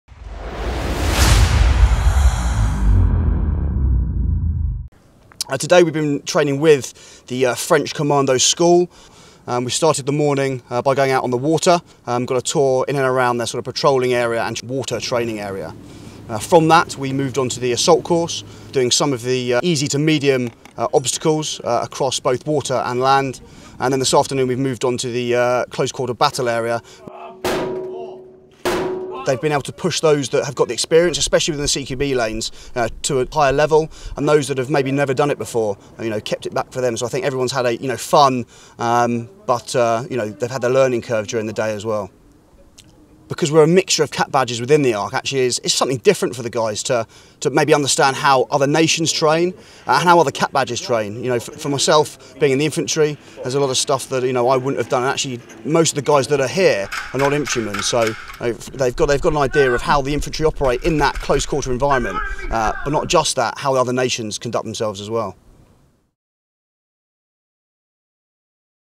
Selva Backstop 2015 French Commando School
During the latter stages of Operation Frankton, the Commandos made their escape with the help of the French Resistance.  To replicate this, the exercise participants of Selva Backstop 2015 have spent the day with the French Commando School at Collioures, near Perpignan conducting low level Assault Course techniques and Close Quarter Battle drills with French weapons firing paint rounds.